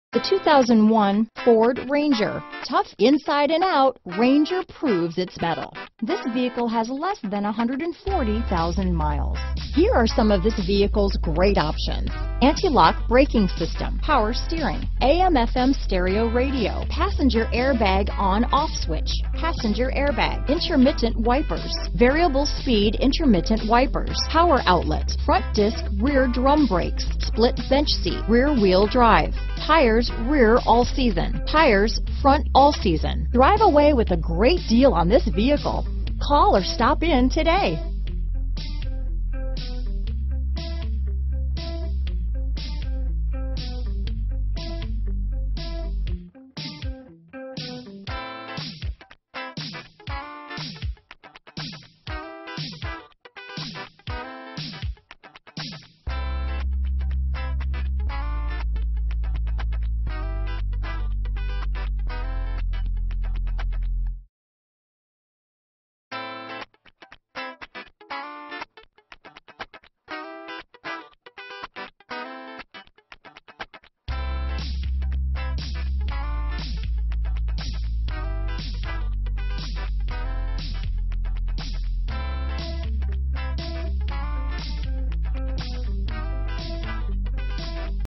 2001 Ford Ranger Louisville, Lexington, Elizabethtown, KY New Albany, IN Jeffersonville, IN 7829A
Oxmoor Ford
100 Oxmoor Ln

2001 Ford Ranger VEHICLE DETAILED, LOCAL TRADE, 2001 Ford Ranger XLT, Ranger XLT, 2D Standard Cab, 2.5L I4 SMPI SOHC, 5-Speed Manual with Overdrive, RWD, Silver Frost Clearcoat Metallic, Dark Graphite, ABS brakes. CARFAX One-Owner.Thank you for visiting Oxmoor Ford -- Louisville's Volume Leader and Number One in Certified Sales.
Rear Wheel Drive,Tires - Front All-Season,Tires - Rear All-Season,Conventional Spare Tire,Power Steering,Front Disc/Rear Drum Brakes,ABS,Intermittent Wipers,Variable Speed Intermittent Wipers,Split Bench Seat,AM/FM Stereo,Power Outlet,Driver Air Bag,Passenger Air Bag,Passenger Air Bag On/Off Switch